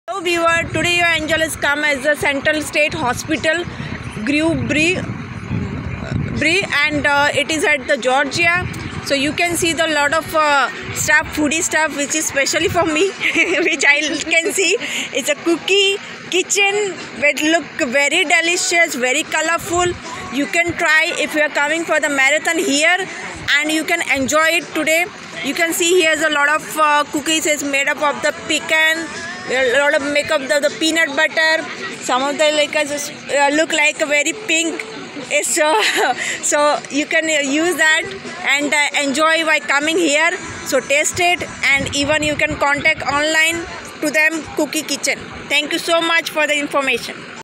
Cookie kitchen booth at Marathon event at Central state hospital, Georgia, USA
Cookie kitchen booth at Marathon event at Central state hospital, Milledgeville, Georgia, USA

varieties of peanut butter and pecan cookies.